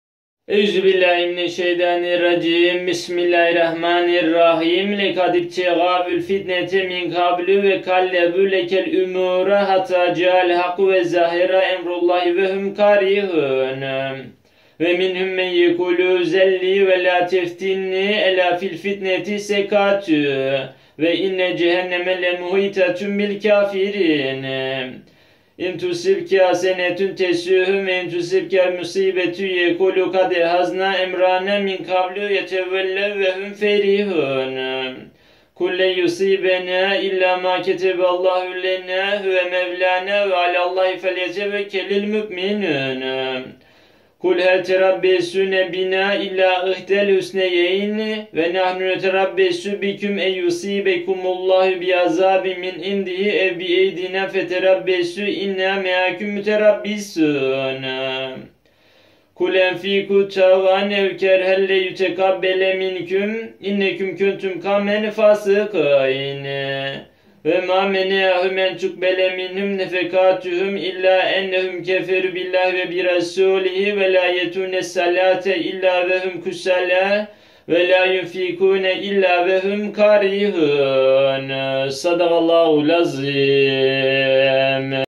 Tevbe Sûresi 48-54.Âyetleri Tilavet(De ki:Allah'ın bizim için yazdığından başkası bize asla erişmez)
48. Andolsun onlar önceden de fitne çıkarmak istemişler ve sana nice işler çevirmişlerdi. Nihayet hak geldi ve onlar istemedikleri halde Allah'ın emri yerini buldu.

49. Onlardan öylesi de var ki: "Bana izin ver, beni fitneye düşürme" der. Bilesiniz ki onlar zaten fitneye düşmüşlerdir. Cehennem, kâfirleri mutlaka kuşatacaktır.

50. Eğer sana bir iyilik erişirse, bu onları üzer. Ve eğer başına bir musibet gelirse, "İyi ki biz daha önce tedbirimizi almışız" derler ve böbürlenerek dönüp giderler.

51. De ki: Allah'ın bizim için yazdığından başkası bize asla erişmez. O bizim mevlâmızdır. Onun için müminler yalnız Allah'a dayanıp güvensinler.

52. De ki: Siz bizim için ancak iki iyilikten birini beklemektesiniz. Biz de, Allah'ın, ya kendi katından veya bizim elimizle size bir azap vermesini bekliyoruz. Haydi bekleyin; şüphesiz biz de sizinle beraber beklemekteyiz.

53. De ki: İster gönüllü verin ister gönülsüz, sizden (sadaka) asla kabul olunmayacaktır. Çünkü siz yoldan çıkan bir topluluk oldunuz.

54. Onların harcamalarının kabul edilmesini engelleyen, onların Allah ve Resûlünü inkâr etmeleri, namaza ancak üşenerek gelmeleri ve istemeyerek harcamalarından başka bir şey değildir.